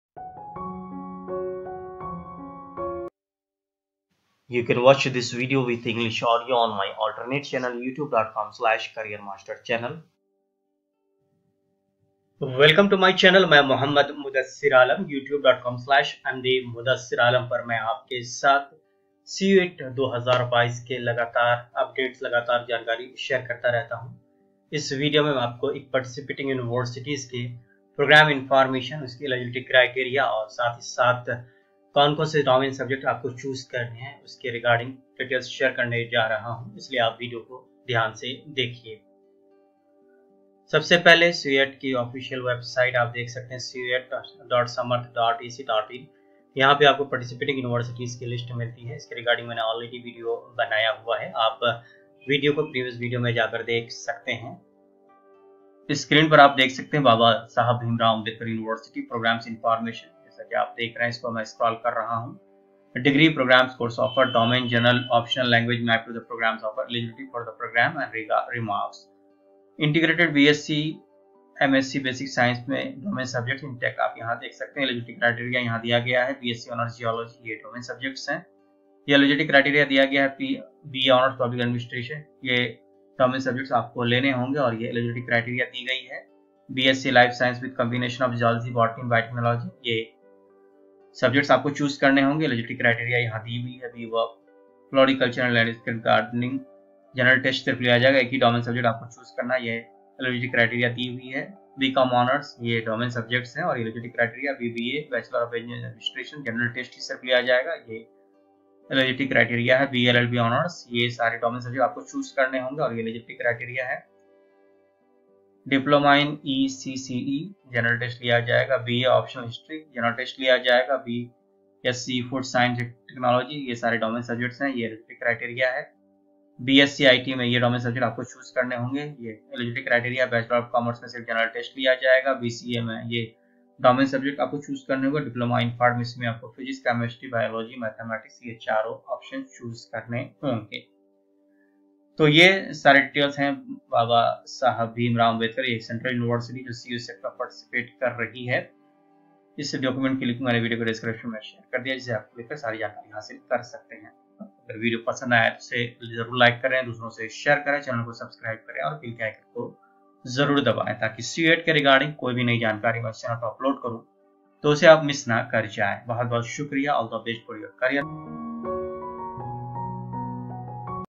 BBAU CUET| Babasaheb Bhimrao Ambedkar University Admission | Programs| Domain Subjects | Eligibility
Babasaheb Bhimrao Ambedkar University  (BBAU) has invited applications for Admission in UG courses through CUET 2022.

Get the details of BBAU Programmes, Domain Subjects and Eligibility Criteria by clicking this